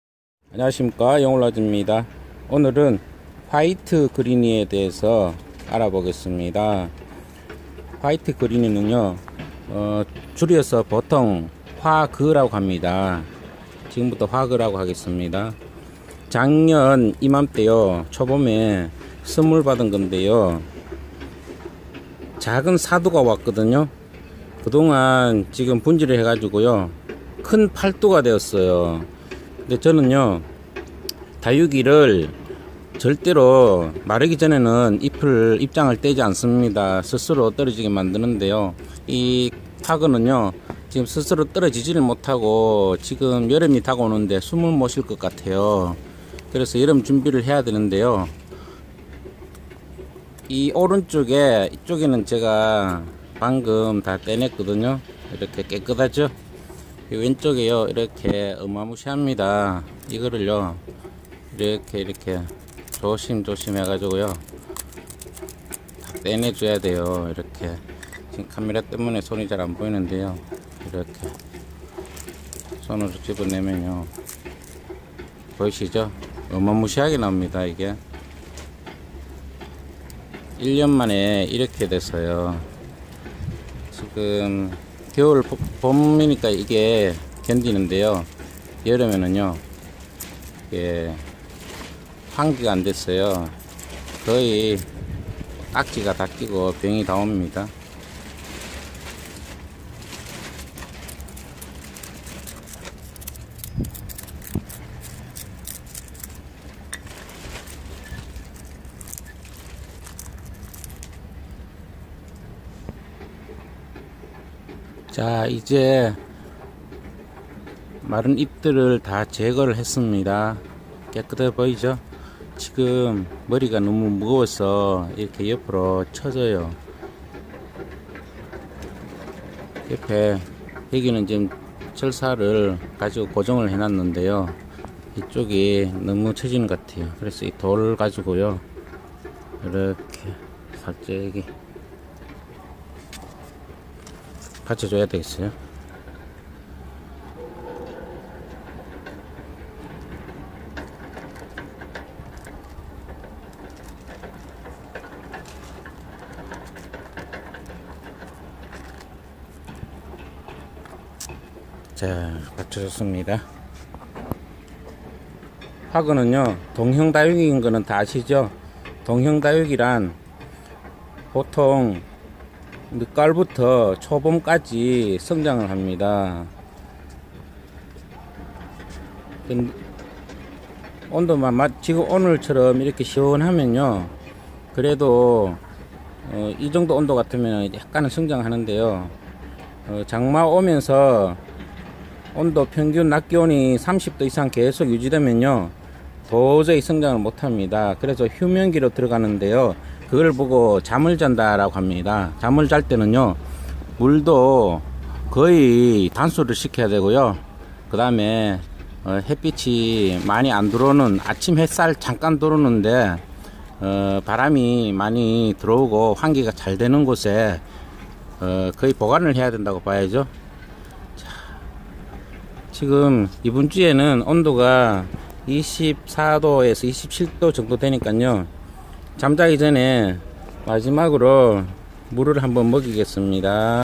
용월아재의 화이트 그리니 잠은 어떻게?
화이트 그리니는 줄여서 보통 화,그라고 합니다.
작년 초봄에 4두로 입양했는데요.
지금 8두로 분지해서
이제 여름이 다가와서 잠잘 준비를 합니다.
영상을 한번 보시죠~~~^^